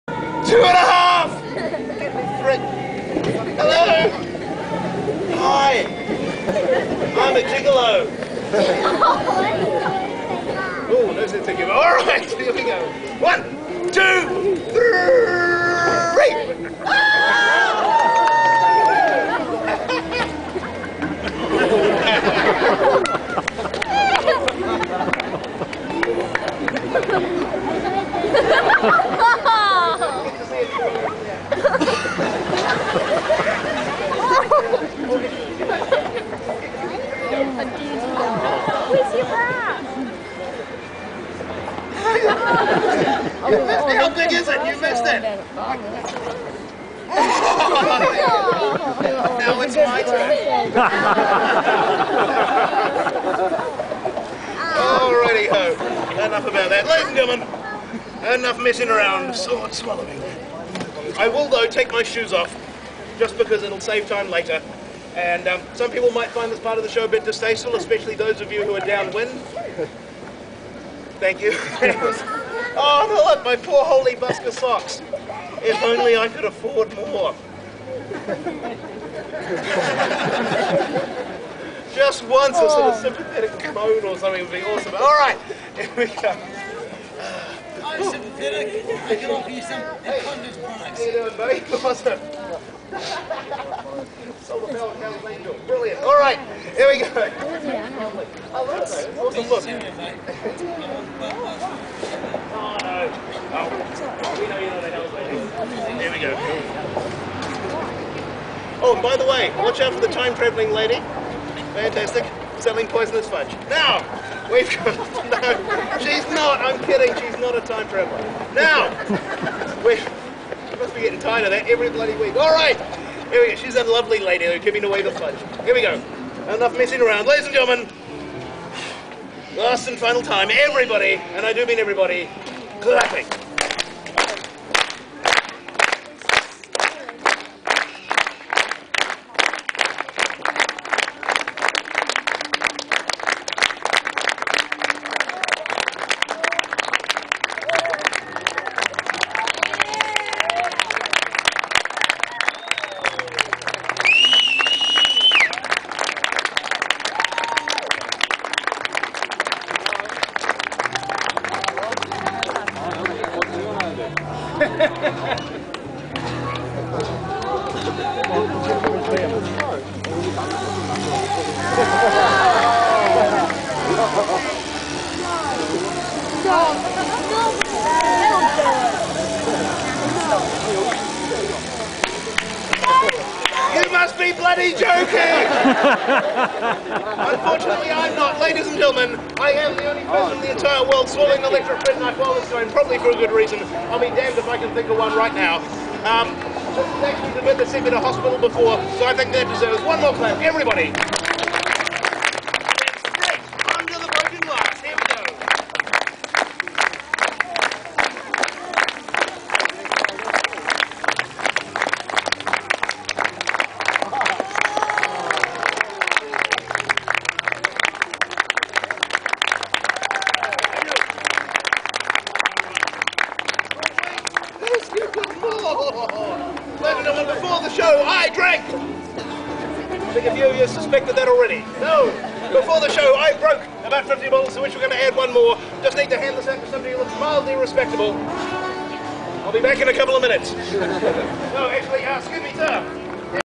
Fat hairy guy swallows 20" sword plus an electric knife!
Street busker, 20 stone, "Hefty Jeff" makes his living by fire eating, sword swallowing, jumping and rolling around on broken glass while cracking jokes and wearing a kilt. His show is done right on the sidewalk where he regularly attracts a crowd of up to 200 spectators. Things don't always go right  - he once had to be taken away in an ambulance after the "electric knife" swallowing stunt went awry.